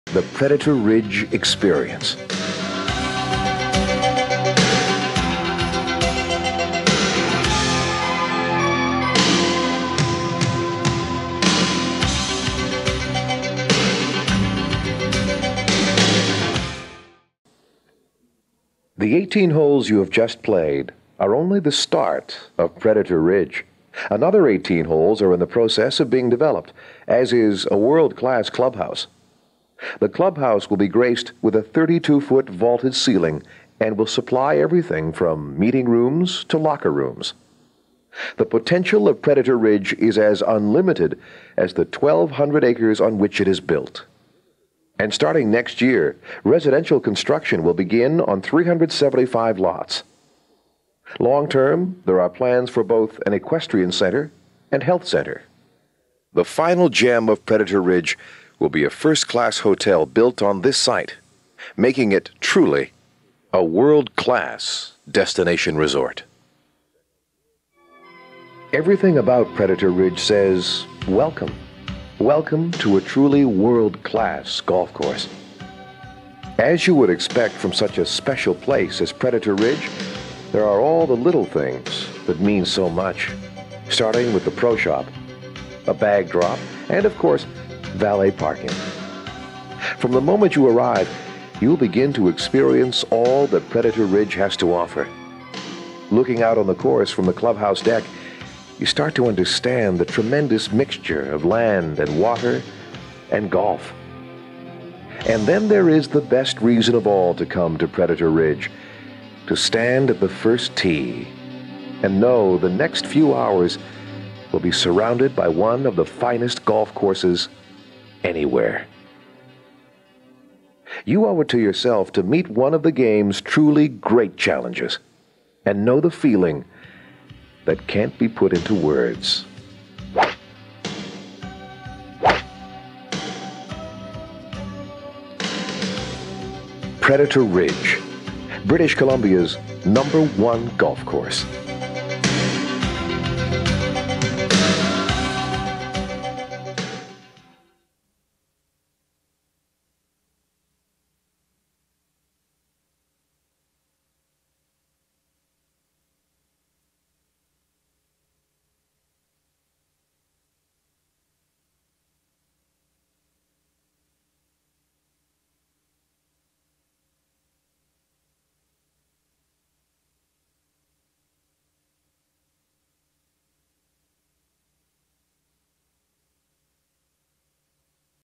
The History of the clubhouse at Predator Ridge Resort
When Predator Ridge opened in 1991, the clubhouse was a double wide trailer that was located where our Member Services building resides now. In the spring of 1993, our current clubhouse was built, featuring a restaurant, members lounge area, cart storage, and a wrap around deck.